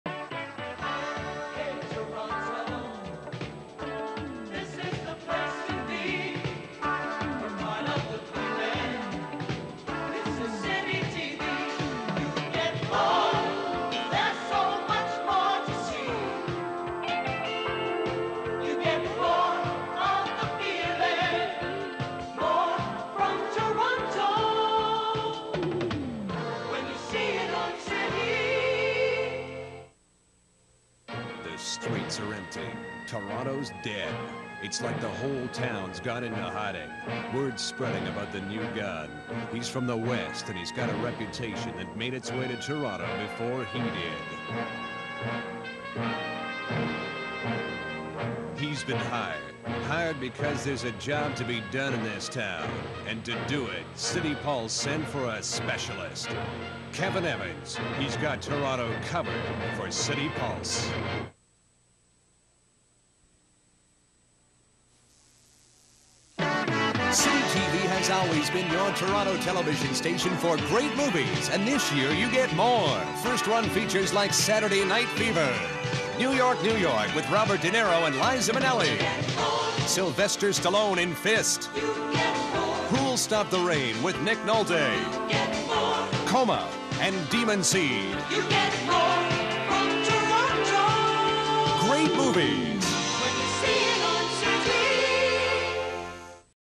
CITYTV PROMOS (1981)
A selection of Citytv promos from 1981.